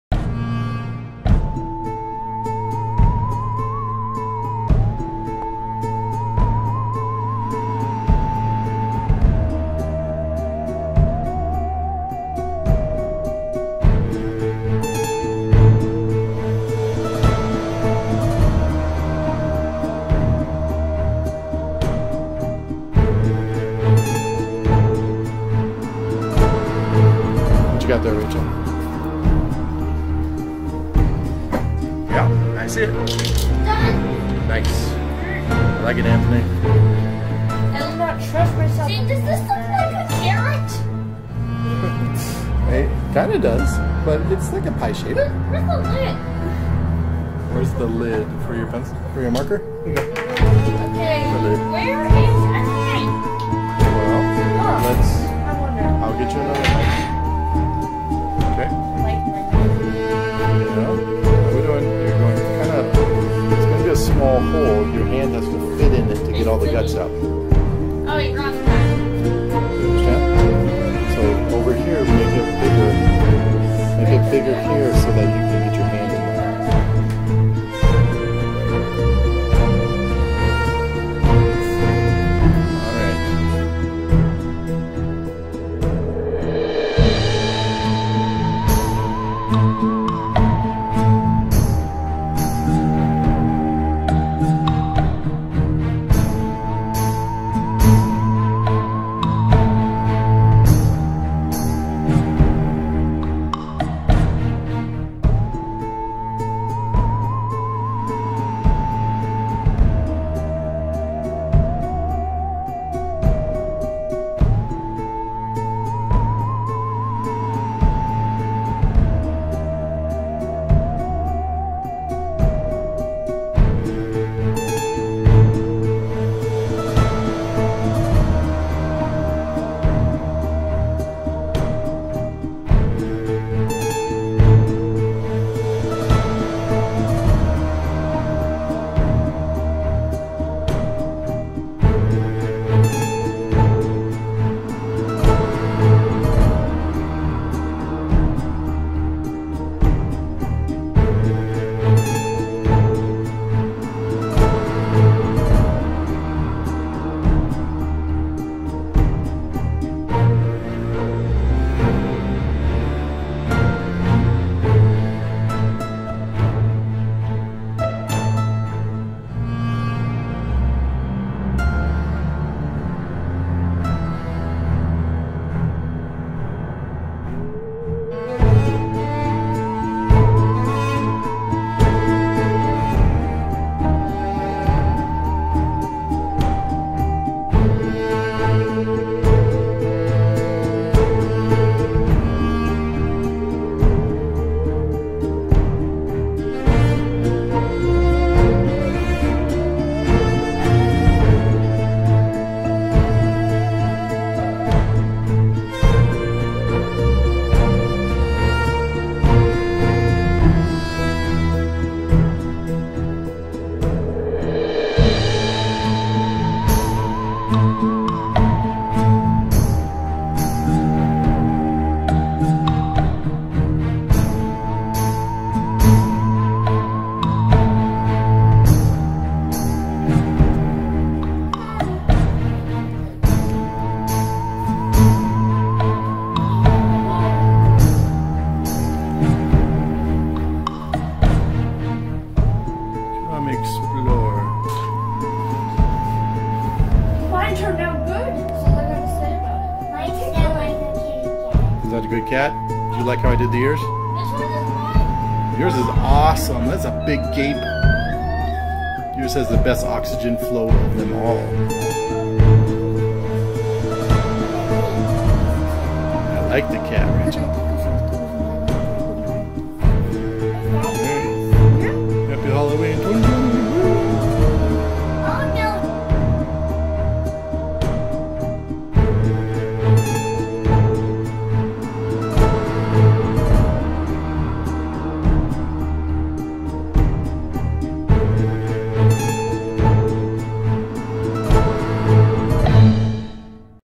Pumpkins 2020
Last minute pumpkin carving on Halloween